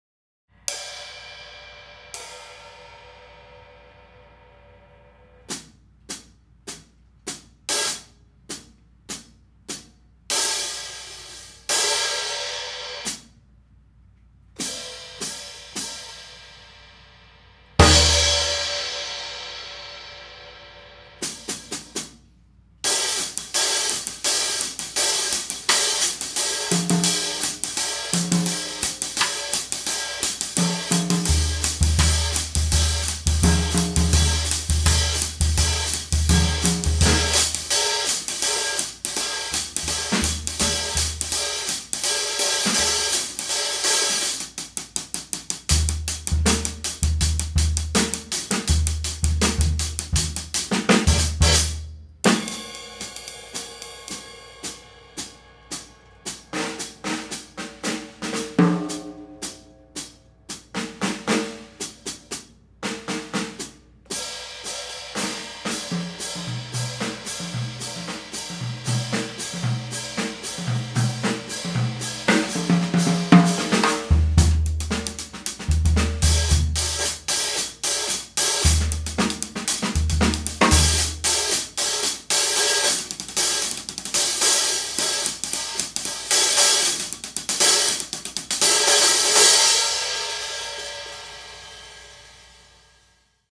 Zildjian 15" K Constantinople Light Hi Hats - 956/1200g (K2716-1071321C)
Zildjian 15" K Constantinople Light Hi Hats available from myCymbal.com.

Other gear used in this video:
• Cymbals (From drummer's L to R): Zildjian 17" K Constantinople Crash Cymbal, Zildjian 20" A Take Five Reissue Ride Cymbal
• Drumheads: Remo Fiberskyn 3 Ambassador Bass Drumhead, Remo Coated Ambassadors